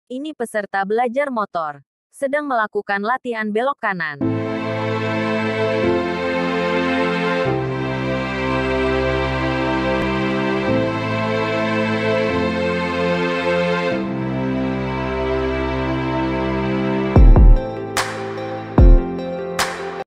Belajar belok
Ini peserta belajar motor , sedang melakukan latihan belok kanan.